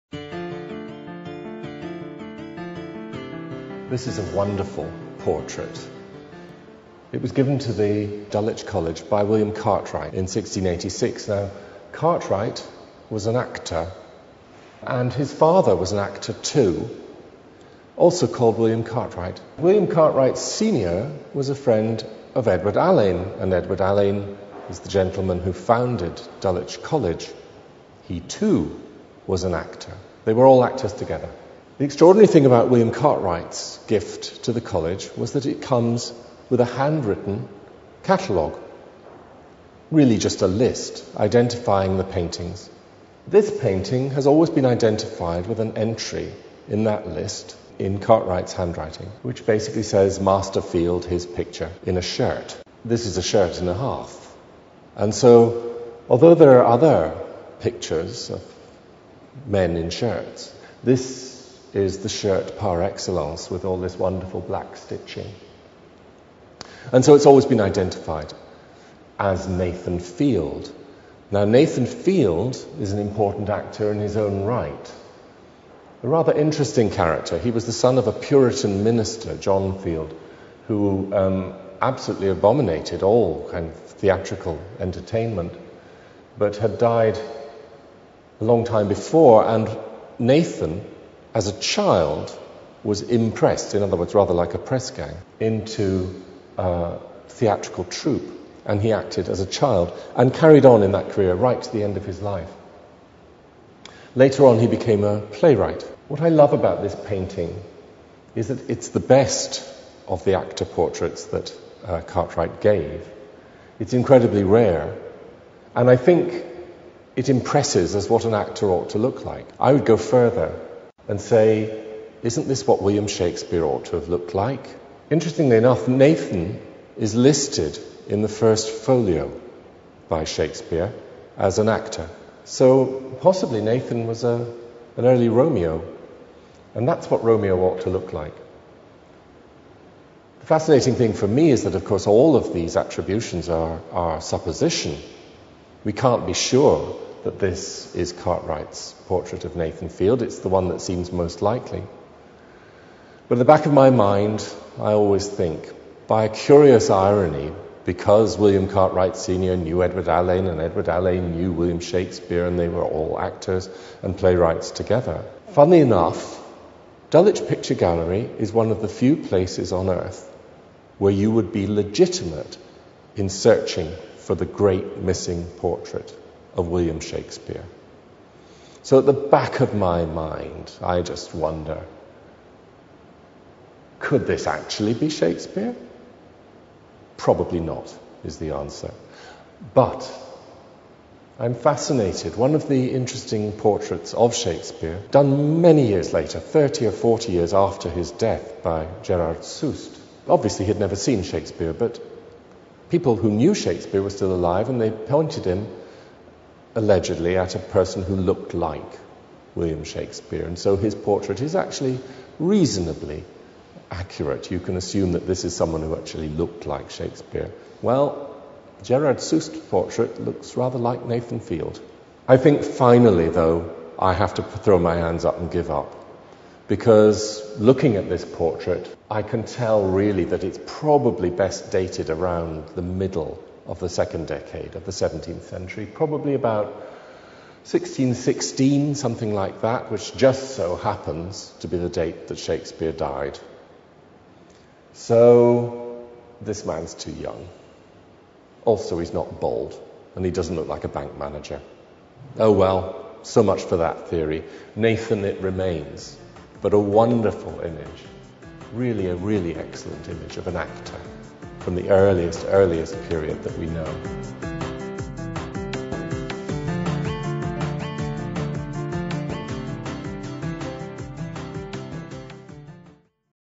Best of British (Nathan Field)
To complement its new and recently published Catalogue of British Pictures, Dulwich Picture Gallery will be exhibiting the best of its British paintings this summer in a captivating exhibition telling the unique story of its British Collection. Masterpieces from the original bequests will be displayed alongside lesser-known paintings bequeathed by collectors other than the Gallerys founders, including a significant group of actors portraits from the time of Shakespeare.